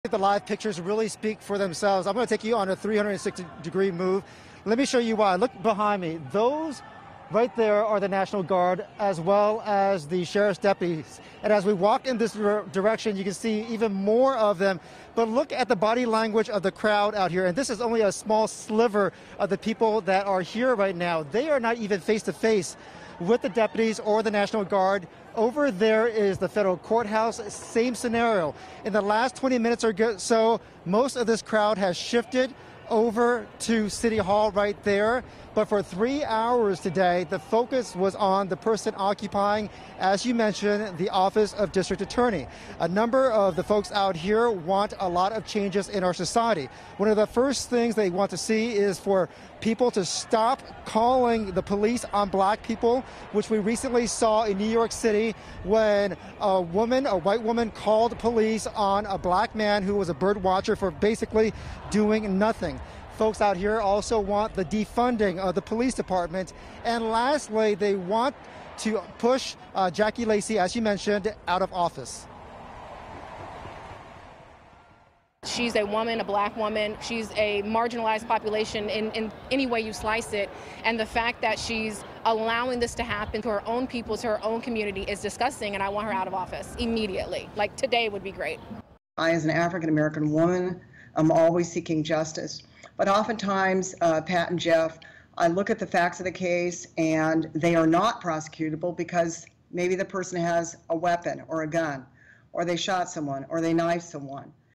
Crowds Gather In DTLA To Demand Justice After George Floyd's Death
Protesters were peaceful in downtown Los Angeles on Wednesday evening in what has been days of demonstrations following the death of George Floyd in Minneapolis. Jeff Nguyen reports.